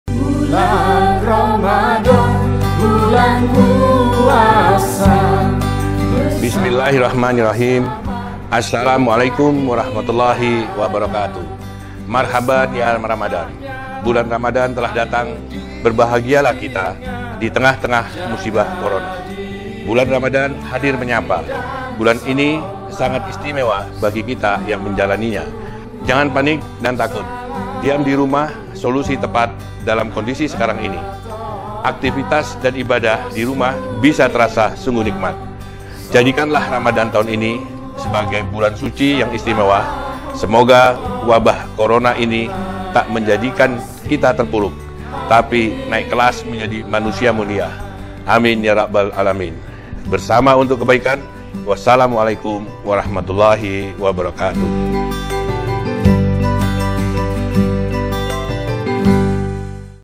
LNA TV, Berkah Ramadhan, Bersama AA LaNyalla Mahmud Mattalitti (Ketua DPD RI)
LA NYALLA ACADEMIA
Episode : Indahnya Ramadhan Bersama Keluarga